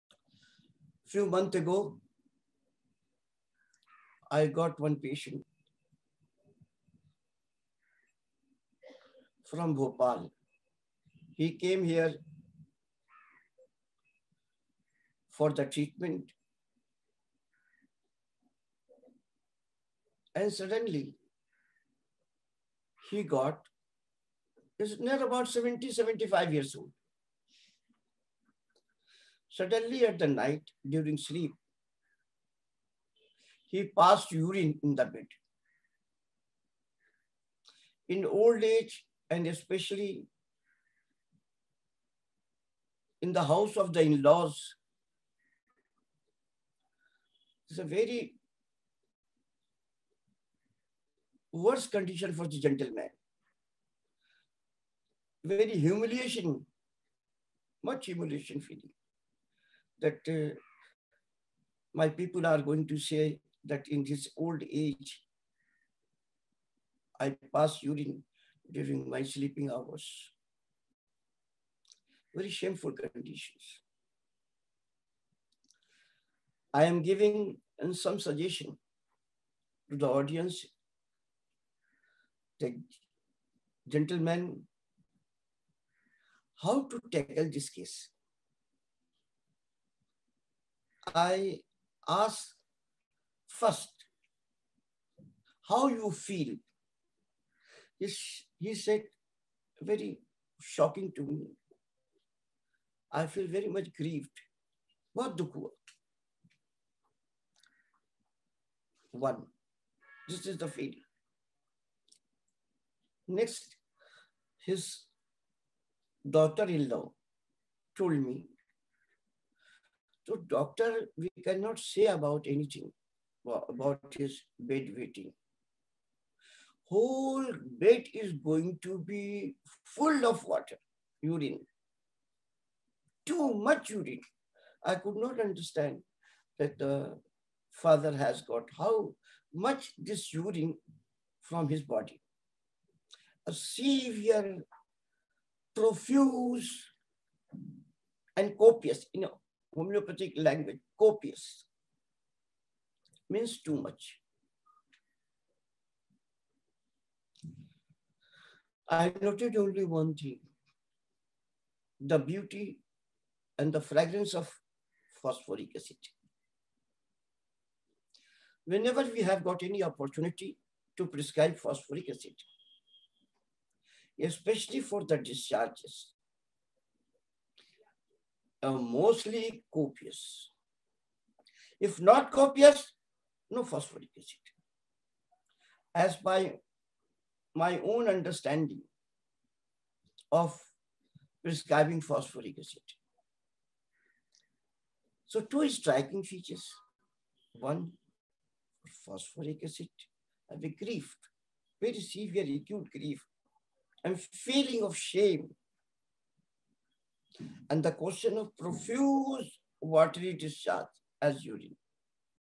Dr L M Khan on Phosphoric acid
Dr L M Khan gives beautiful glimpses of the indication of Phosphoric acid in an excerpt from his lecture at Homoeocon